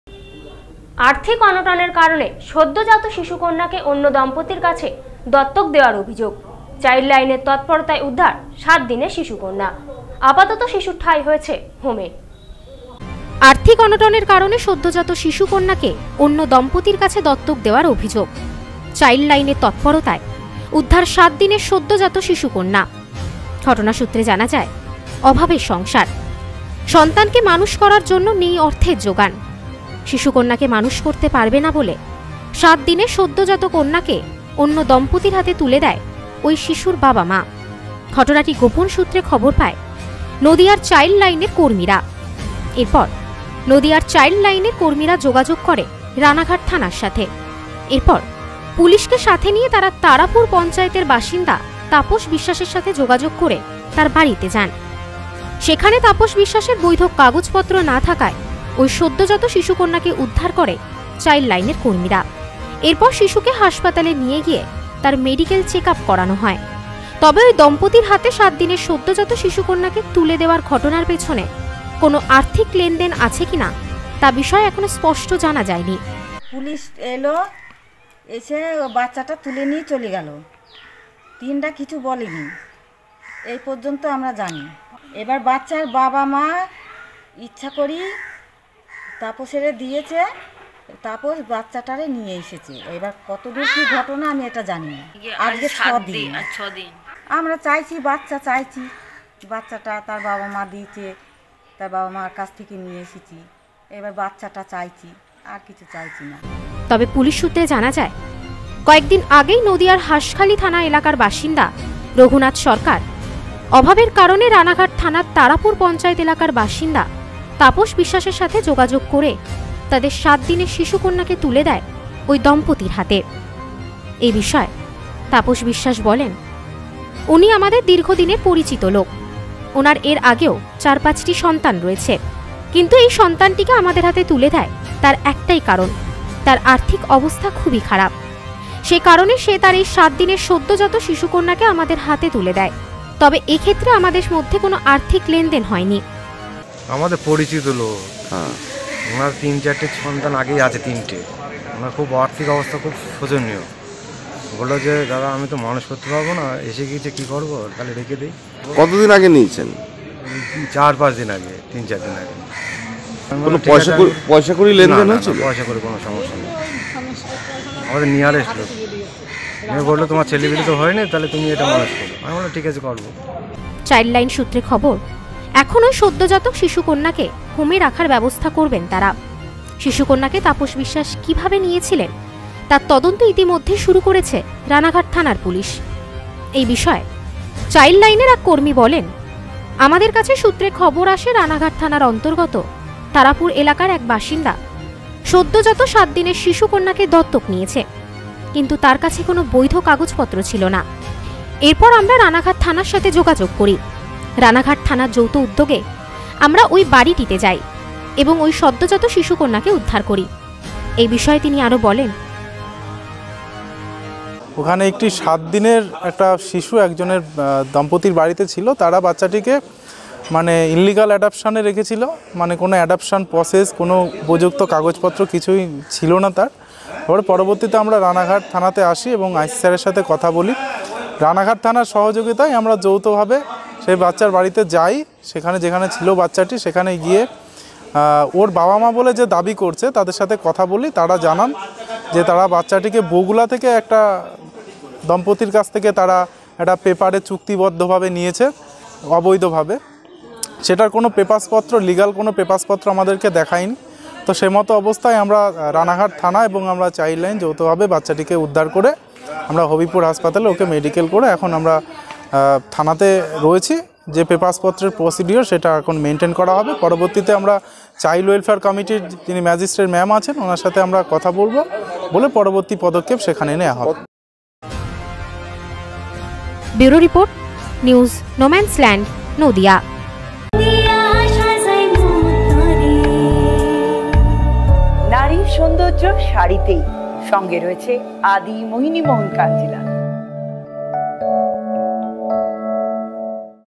আর্থিক অনটনের কারণে সদ্যোজাত শিশুর কন্যা কে অন্য দম্পতীর কাছে দত্তক দেওয়ার অভিযোগ।
আর্থিক অনটনের কারণে সদ্যোজাত শিশুর কন্যা কে অন্য দম্পতীর কাছে দত্তক দেওয়ার অভিযোগ। চাইল্ড লাইনের তৎপরতায় উদ্ধার ৭ দিনে শিশু কন্যা। আপাতত শিশুর ঠাঁয় হয়েছে হোমে।